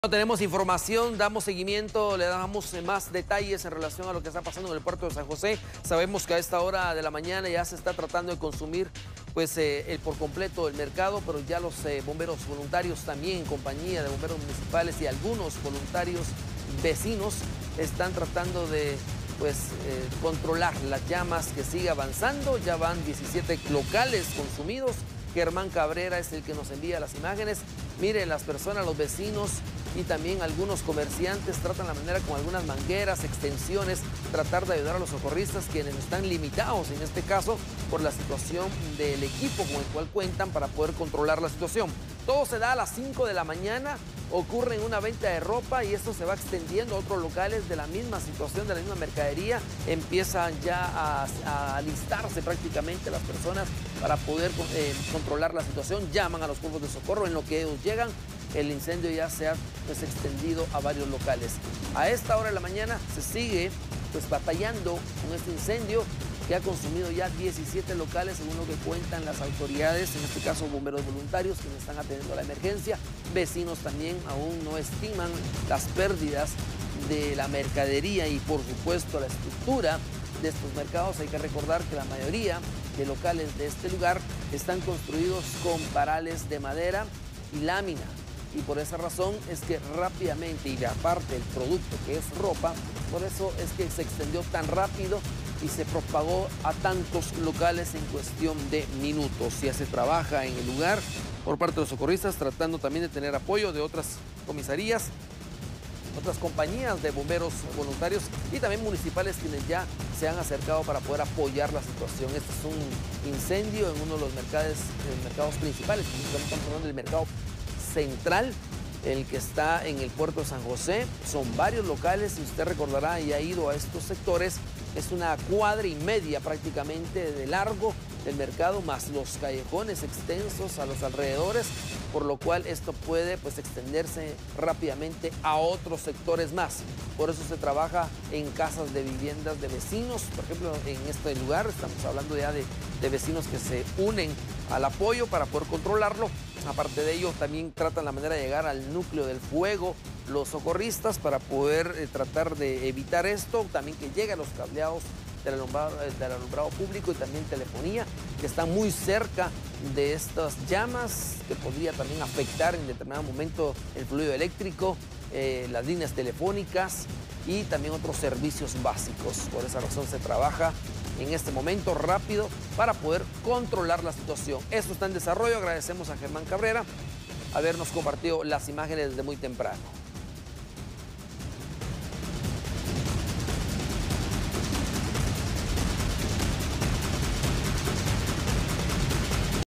Vecinos y socorristas se unen para controlar incendio en Puerto de San José
Columnas de humo emanan del lugar.

En las imágenes se puede ver el momento en que locales del Mercado del Puerto de San José prenden en llamas. Visita nuestro sitio